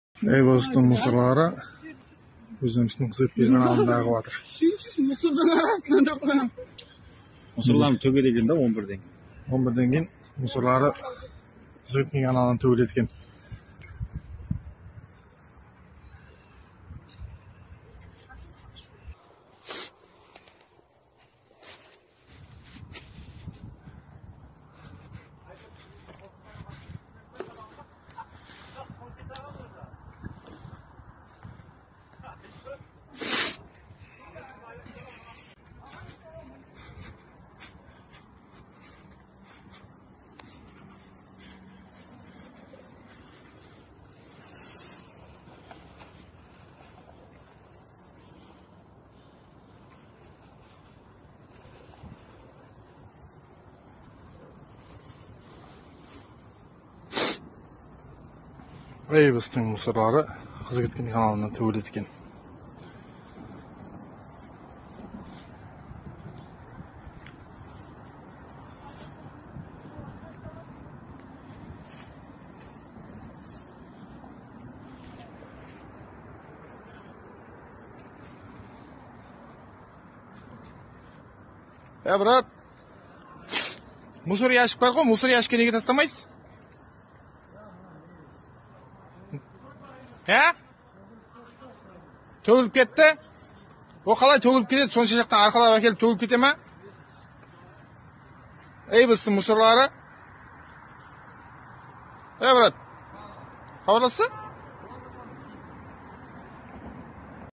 ❗️ EVOS тың шығындылары «Қызкеткен»ге тасланбақта
Мәлим болыўынша, Дослық (Қызкеткен) каналы бойында жайласқан «EVOS» аўқатланыў орны шығындыларын кешки ўақытта каналға ағызып жибермекте.

Видеода «Дослық» каналына «EVOS» маркасы астындағы шығындылардың каналға тасланып атырғанын көриўиңиз мүмкин.

Жағдай шеңберинен рәсмий шөлкемлерден анық жуўап күтемиз.